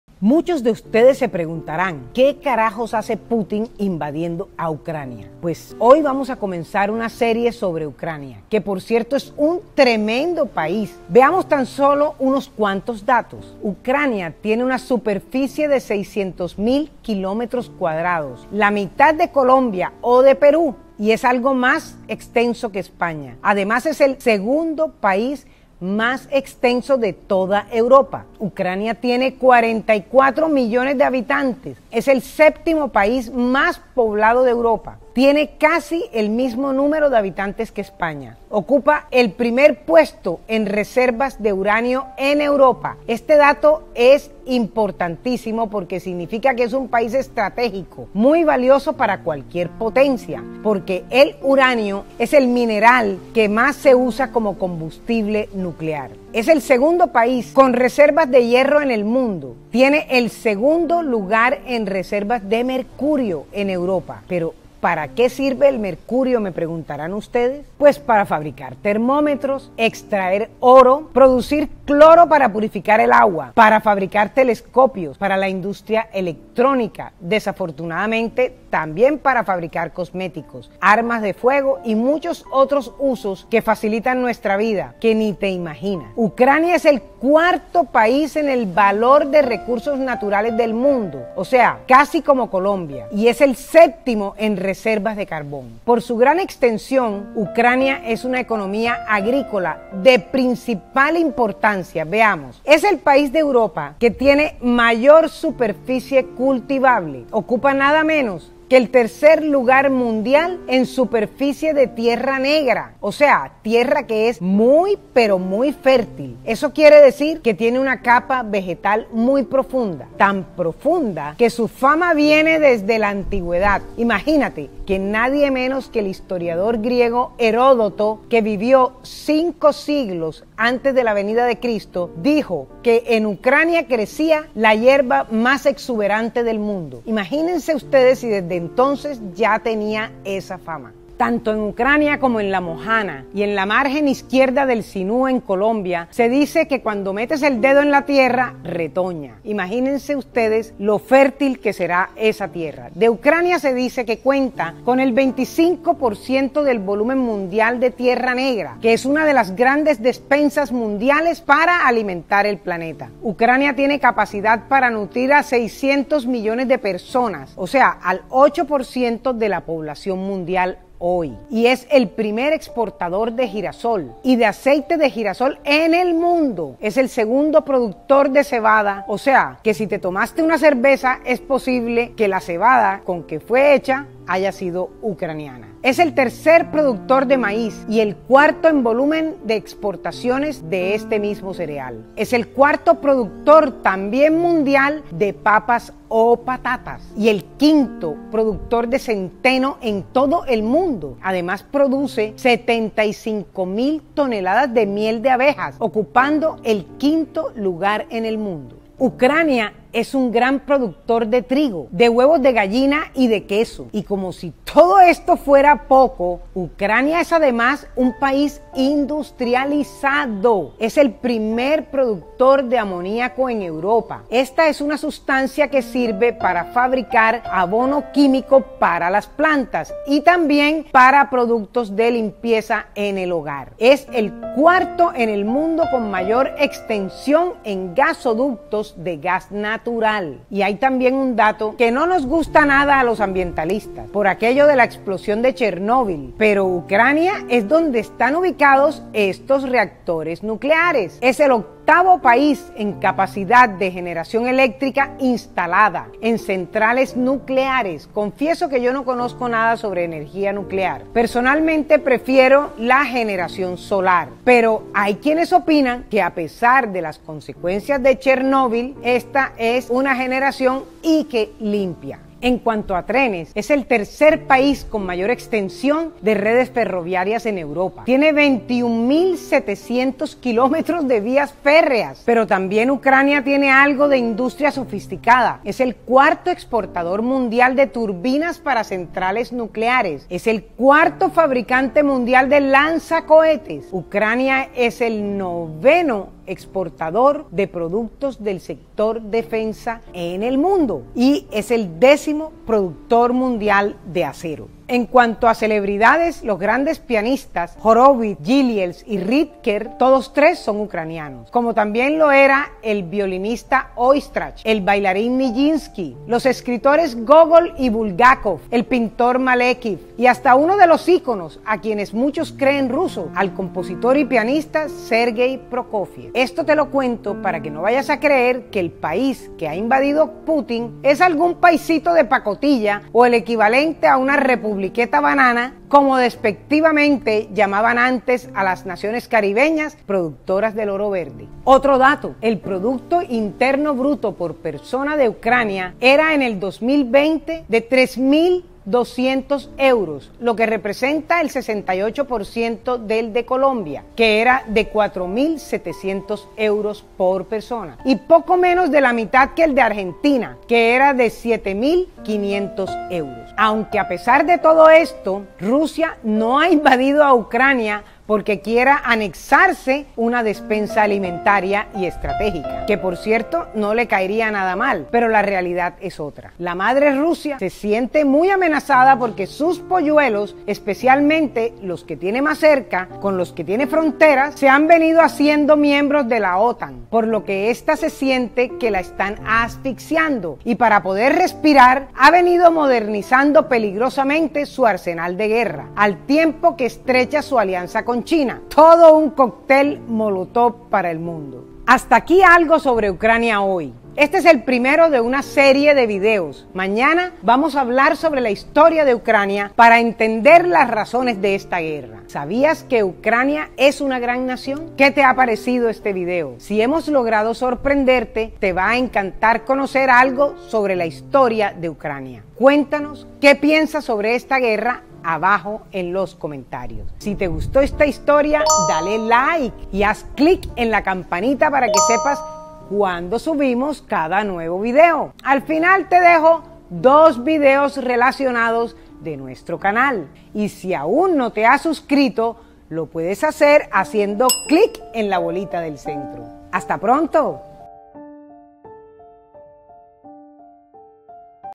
Ucrania en cifras: el país q ha invadido Putin es mucho más grande de lo que se piensa en América❗️
Aquí te mostramos algunas cifras sobre Ucrania, correspondientes a 2020.

Créditos:
Dirección, guión y presentación: Adelina Covo
Producción y edición: Antonio Luis Ángel Covo